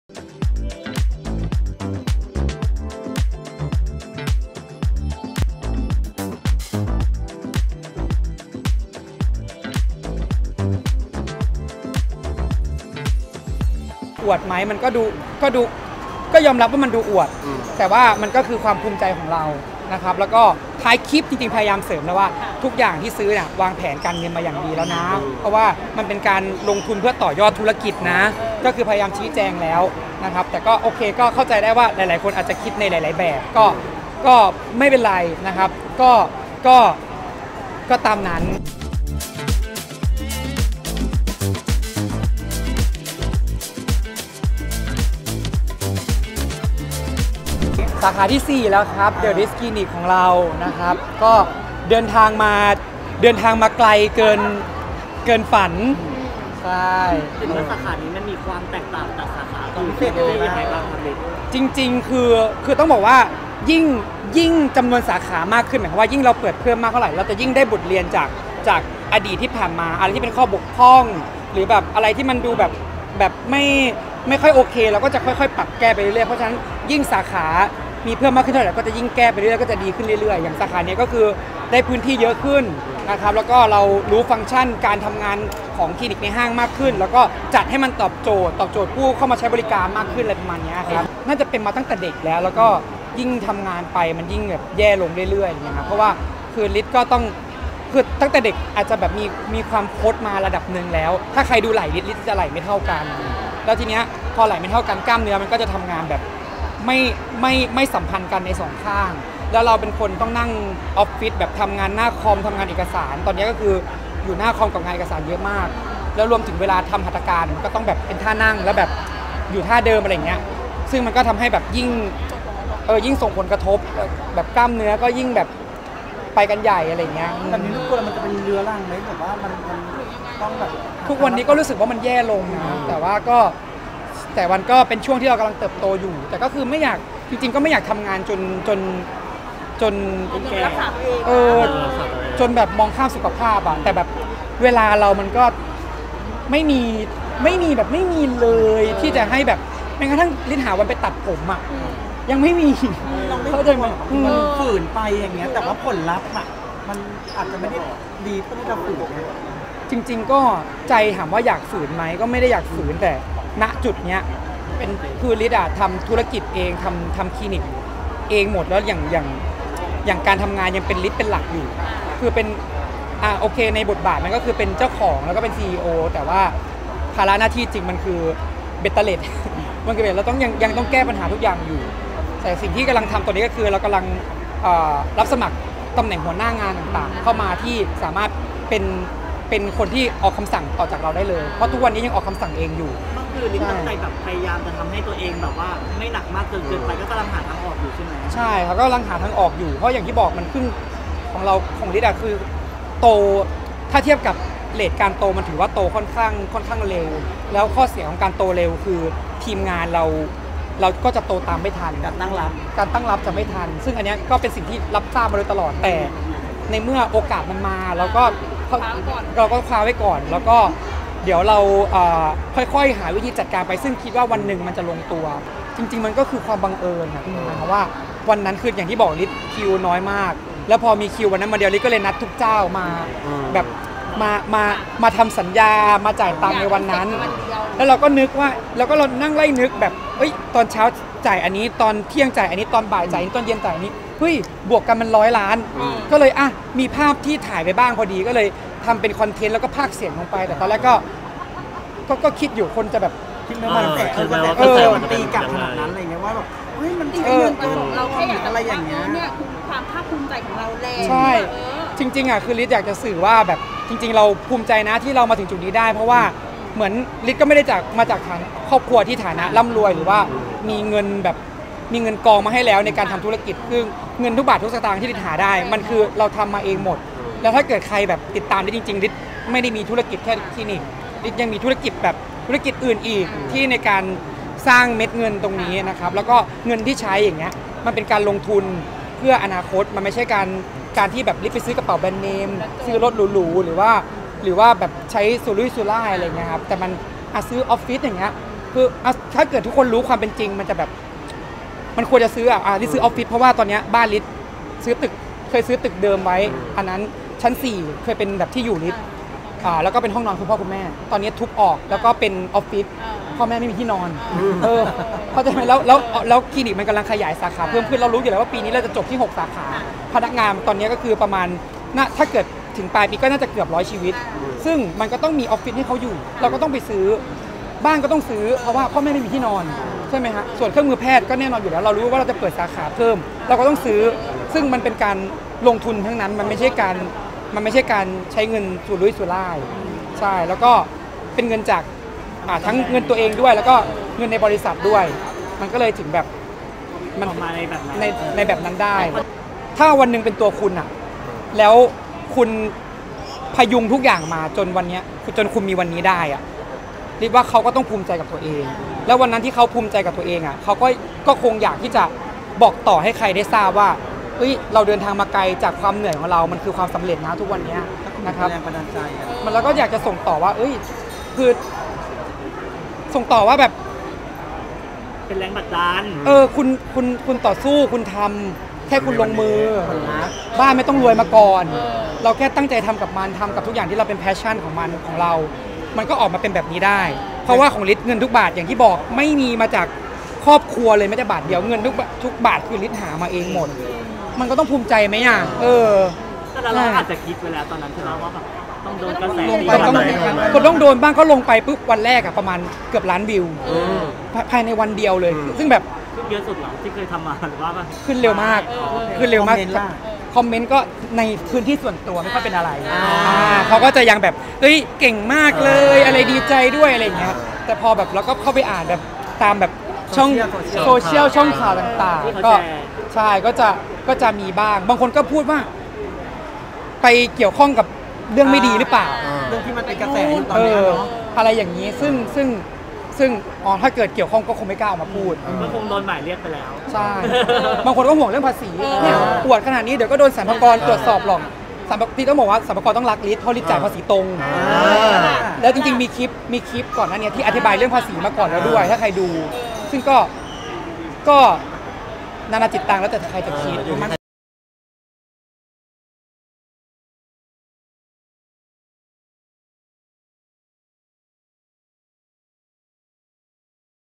ริท เรืองริท เปิด THE RITZ สาขา 4 แจงใช้เงิน 100 ล้านใน 1 วัน
ความจริงเเล้วนั้น มันคือความบังเอิญ ที่วันนั้นมีเวลาน้อยมาก เราเลยนัดทุกเจ้ามาทำสัญญาพร้อมกันในวันเดียว จริงๆ สิ่งที่เราอยากจะสื่อคือว่าความภูมิใจในตัวเองที่มาถึงจุดนี้ได้ เพราะเราสร้างเเละหามันมาด้วยตัวเอง ยอมรับว่ามันอาจจะดูเหมือนอวดรวย แต่มันก็คือความภูมิใจของเรา